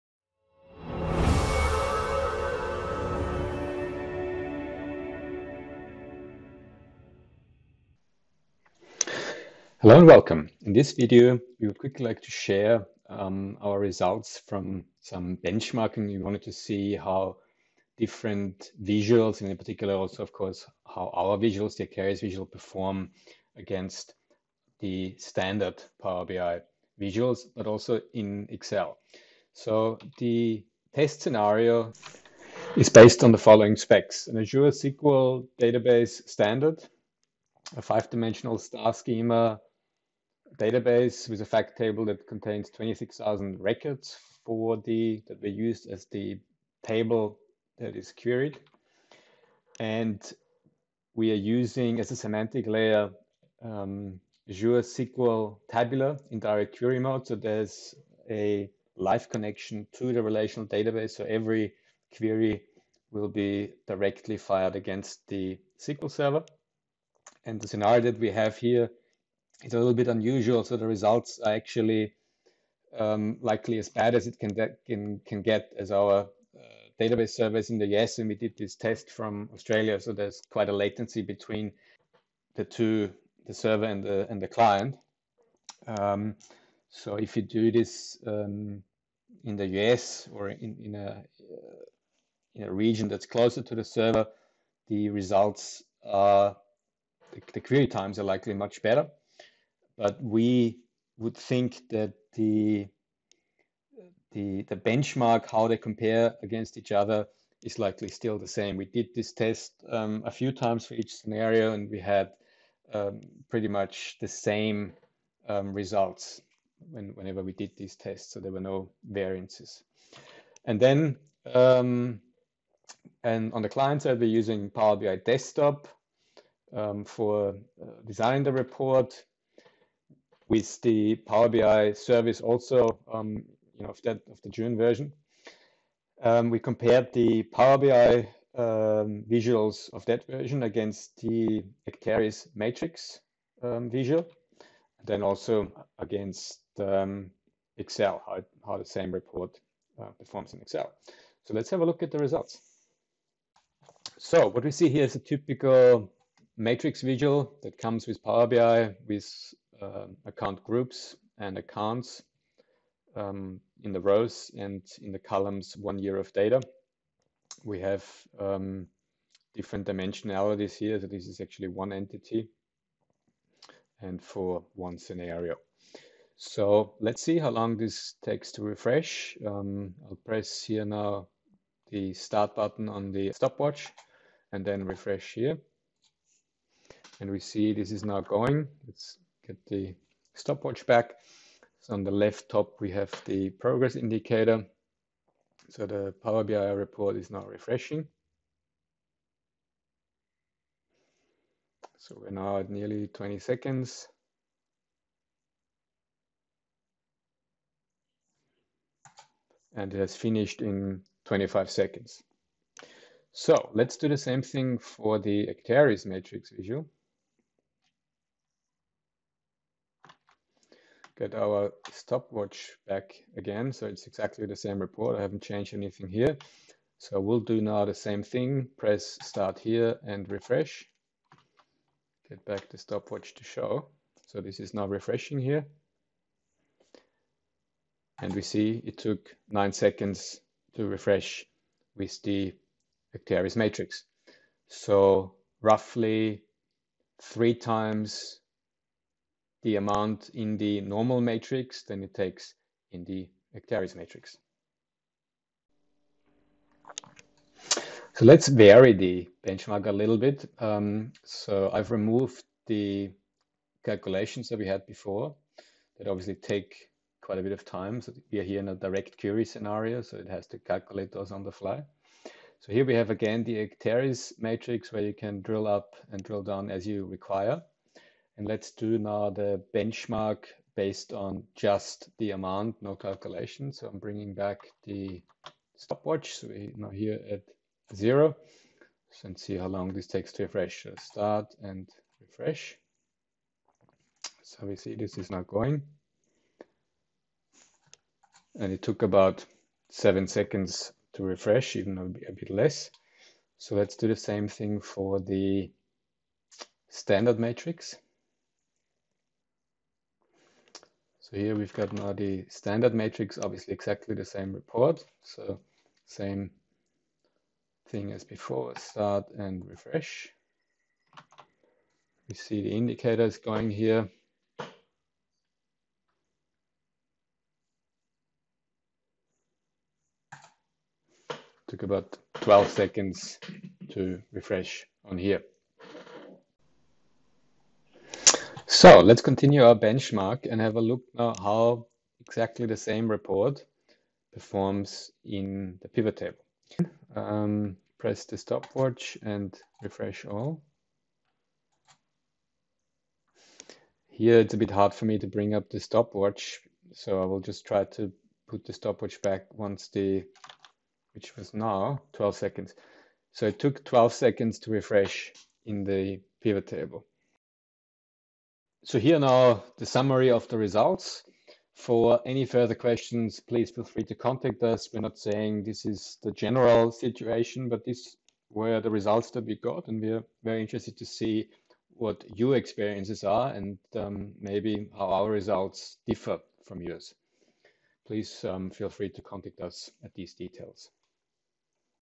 Power BI Matrix vs Excel vs Acterys Matrix a comparison
Benchmark of full refresh times with Power BI Matrix visual /Acterys Matrix visual and Excel Pivot Table using an SSAS Tabular source in Direct Query mode alongside an Azure SQL database.